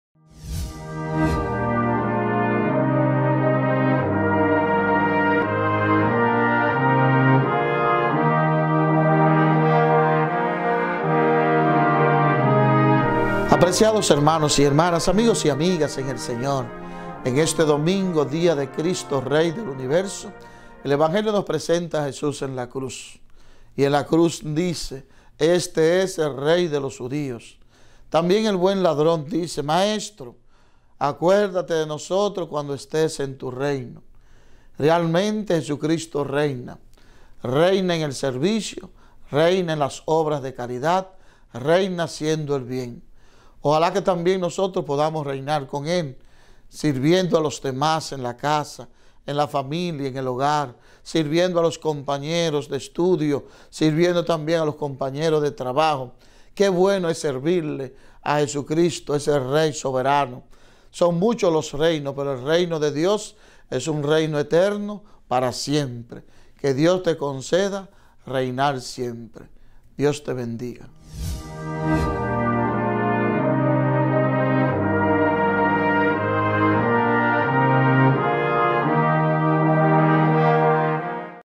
Reflexión Dominical del padre Kennedy Rodriguez - 24 de Noviembre 2019
Reflexionemos en las lecturas de la fiesta de Cristo Rey del Universo.